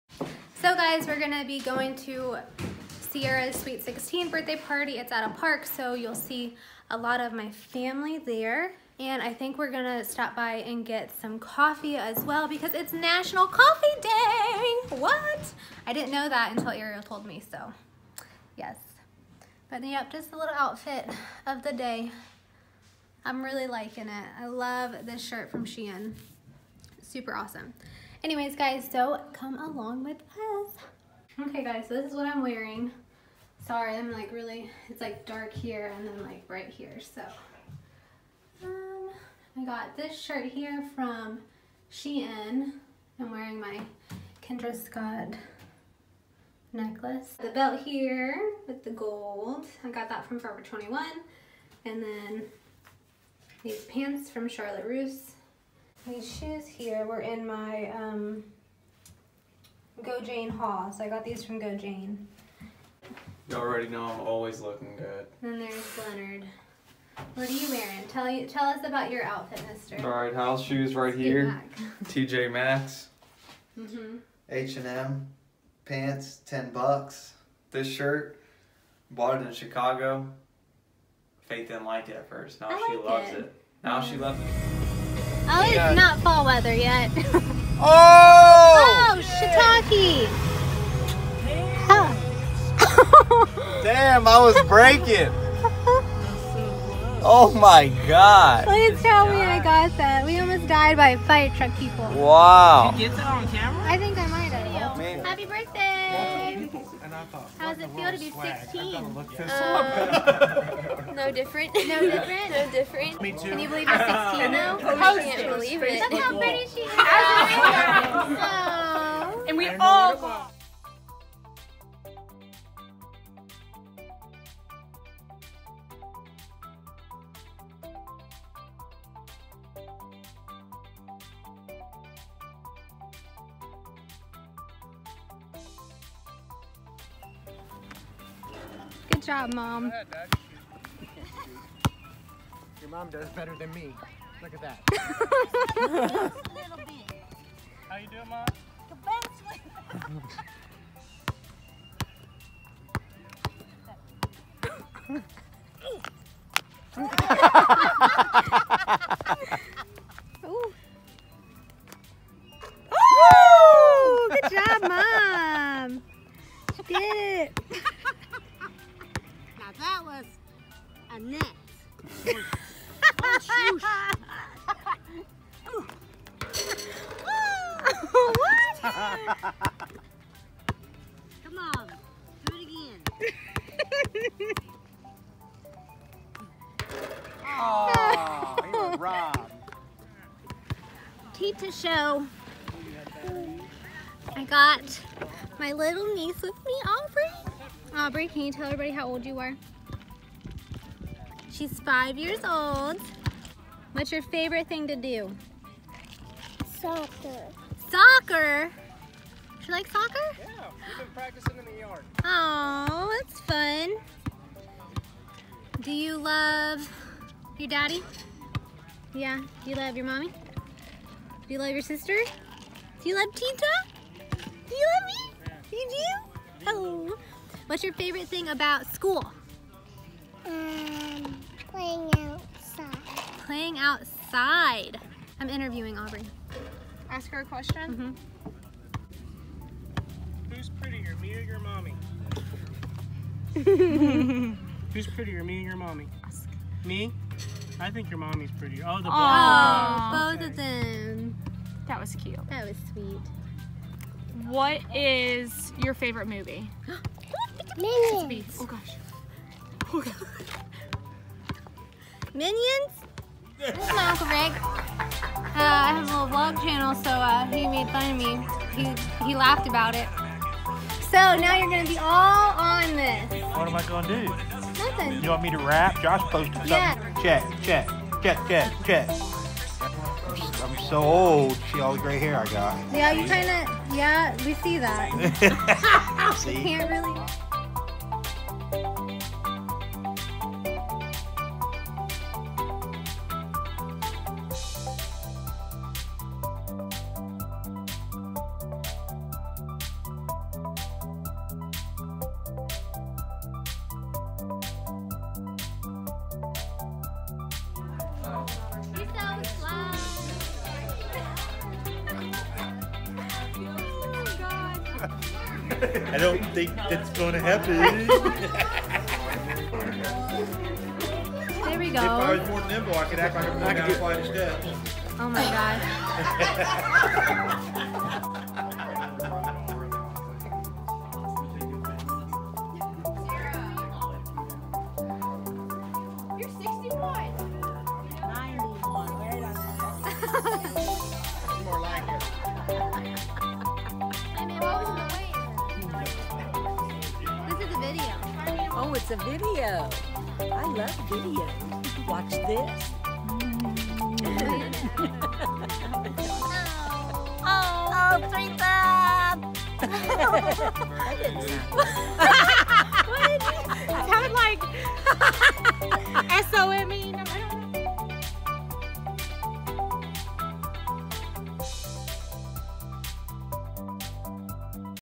Ciera’s 16th Birthday
Ciera’s birthday party & spending time with our family